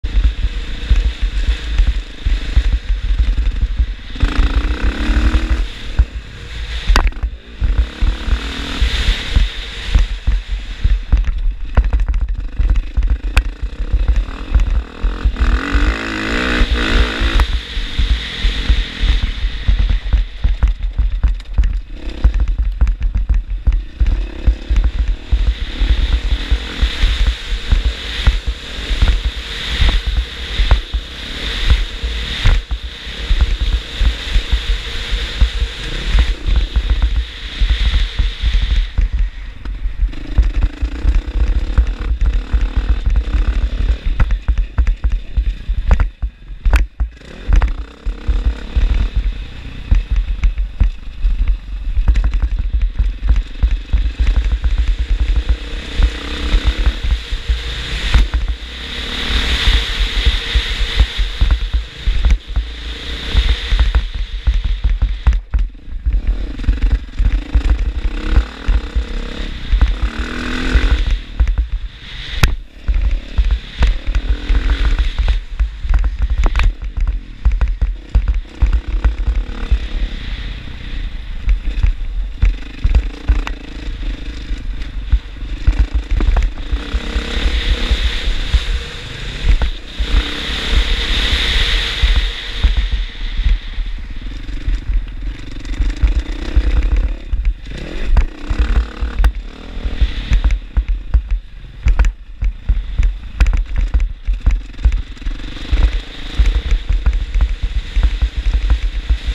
Starting back on the mx track again! (Razor's Edge Mx).
4/13/2017.
Still taking it easy. Trying to get to racing mode. Letting the broken leg get use to the jumping and strain that comes with impact. Having a good time getting back too it!